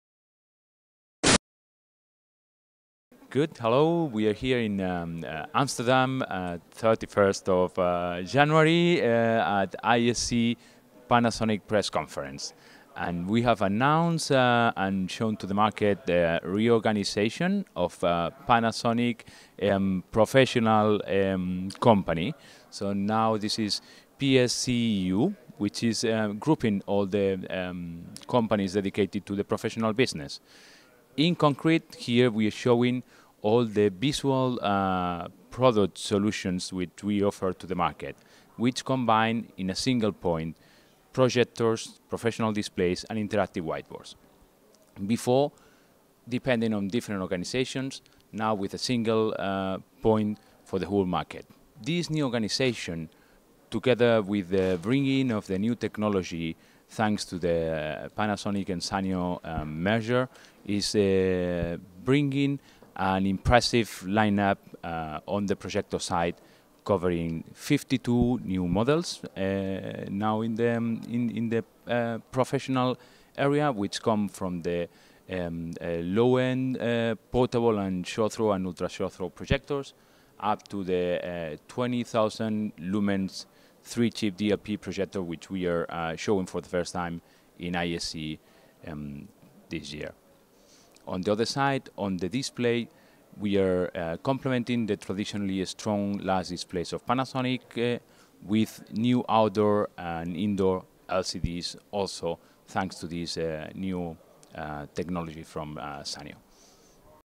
ISE 2012 - Intervista a Enrique Robledo di Panasonic
Enrique Robledo, European Marketing Manager Professional Plasma Business Unit Europe Panasonic, in occasione della conferenza stampa tenuta al Nemo qui ad Amsterdam, racconta gli ultimi interessanti sviluppi per il settore professionale di Panasonic, anche in seguito all'acquisizione di Sanyo (in inglese).

Speciale ISE 2012 (31 Gennaio - 2 Febbraio 2012, Amsterdam RAI, NL) - Tutte le novità in diretta dall'Integrated Systems Europe, la fiera più importante a livello europeo per i settori dell'AV professionale e dell'integrazione di sistemi.

ISE 2012 - Interview to Enrique Robledo of Panasonic

Enrique Robledo, European Marketing Manager - Professional Plasma Business Unit Europe at Panasonic, on the occasion of Panasonic press conference held at Amsterdam'Nemo , talks about latest development by Panasonic for the professional industry, also considering the Sanyo takeover.